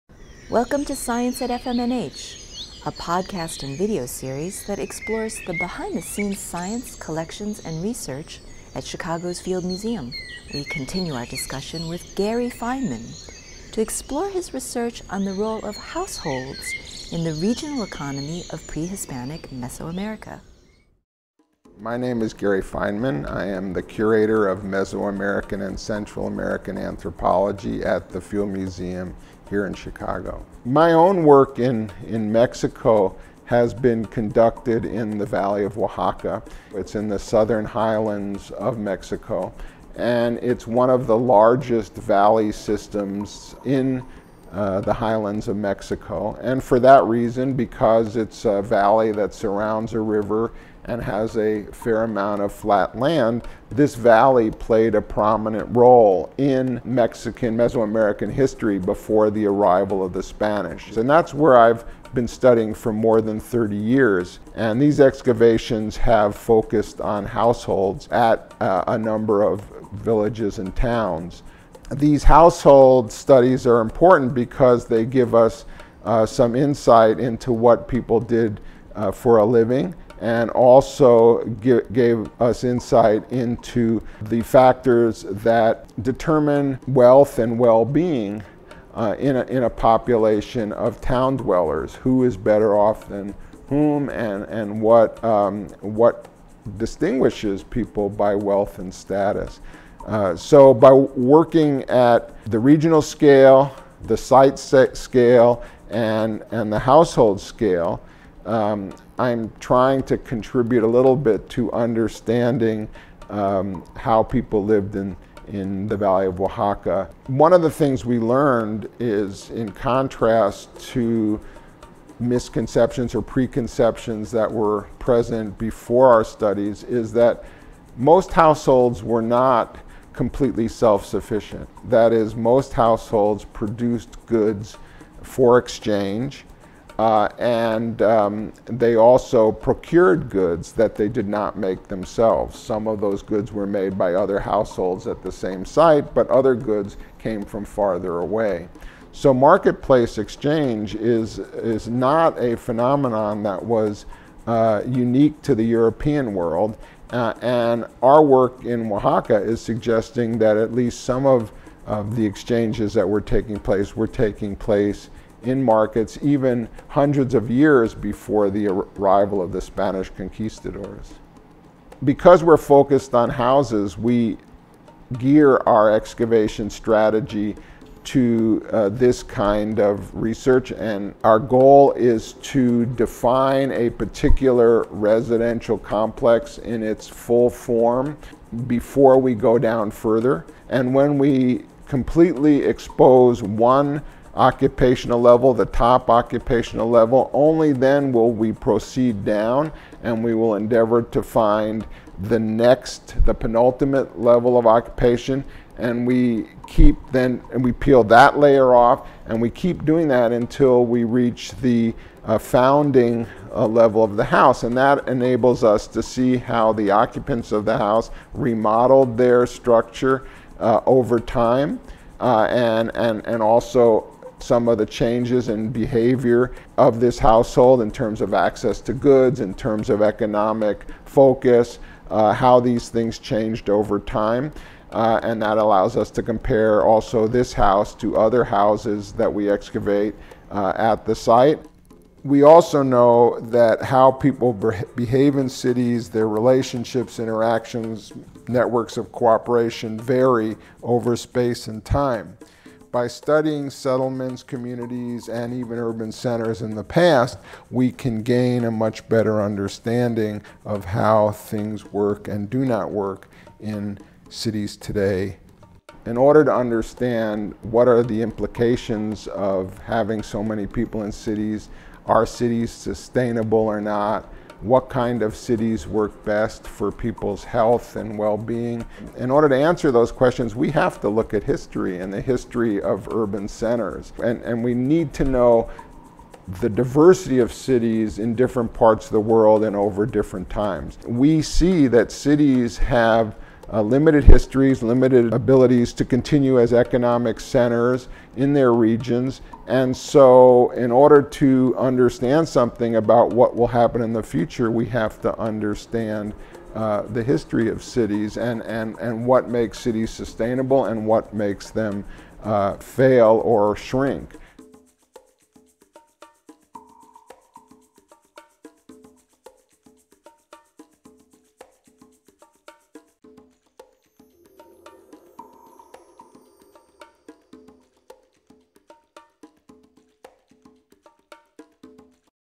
Science at FMNH - Roles of Households in Prehispanic Mesoamerica
We continue our discussion on urbanization with Dr. Gary Feinman to explore his research on the role of households in the regional economy of prehispanic Mesoamerica.

Science at FMNH is a podcast and video series that explores the behind-the-scenes science, collections and research at Chicago's Field Museum.